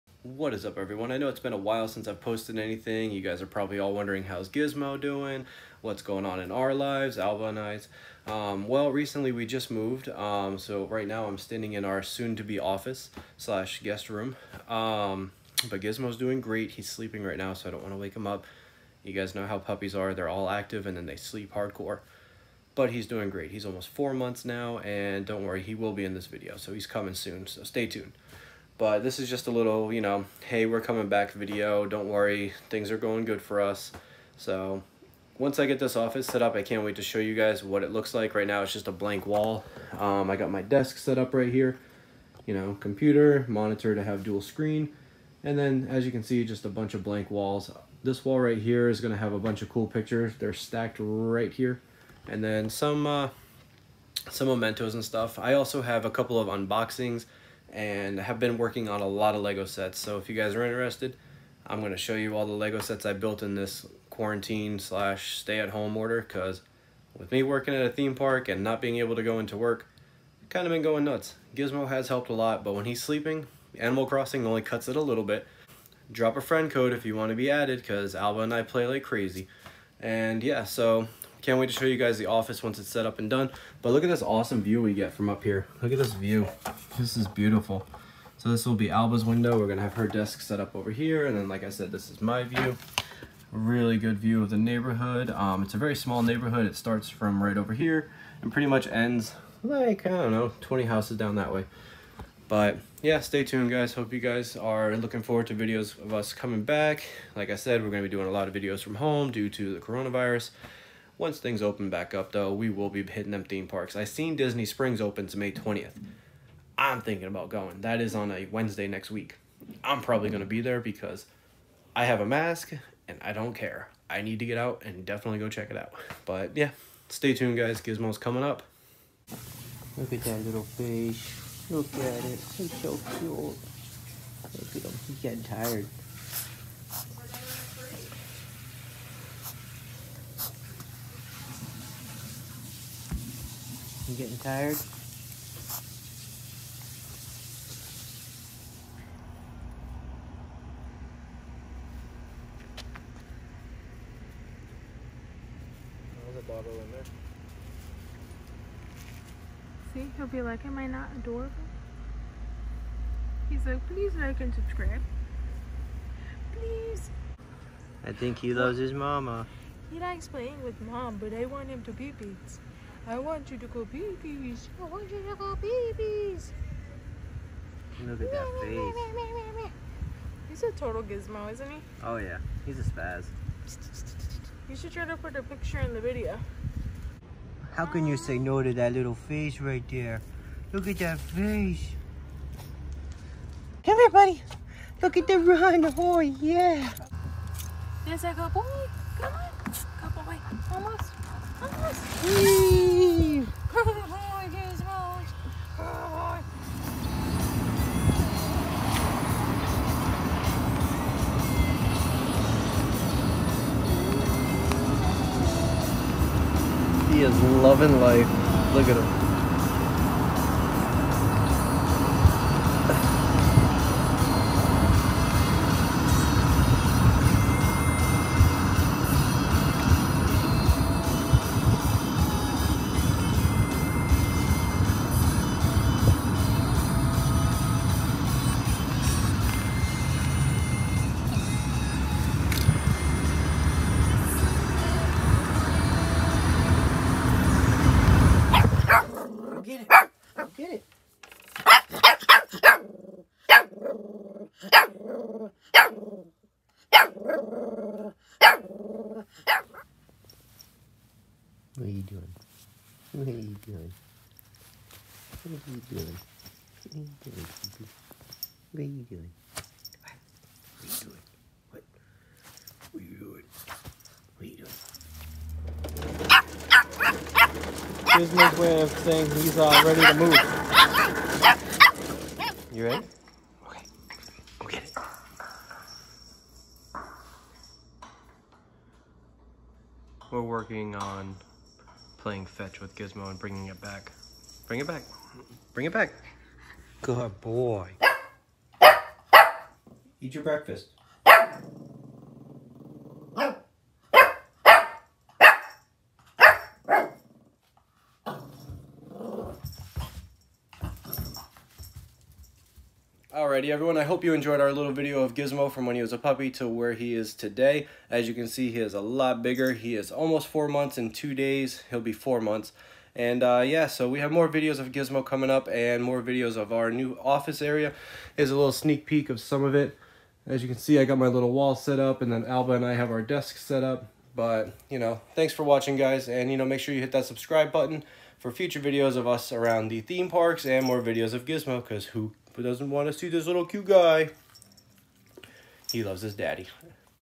WE’RE BACK!
Here’s a little fun video of Gizmo Growing up. more to come but we just wanted to post a comeback video!